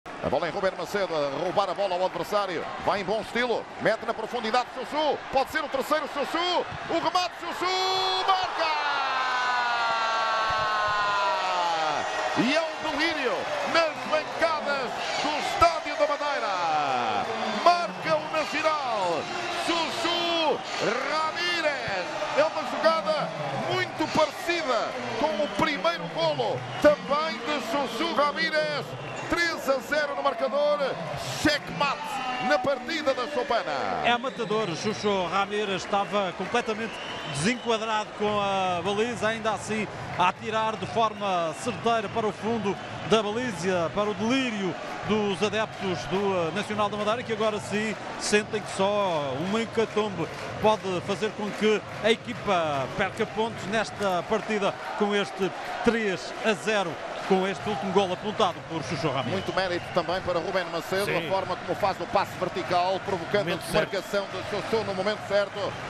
Golo Chuchu Ramírez: Nacional (3)-0 FC Porto B - Liga Portugal SABSEG | sport tv
CD Nacional: Golo de Chuchu Ramírez!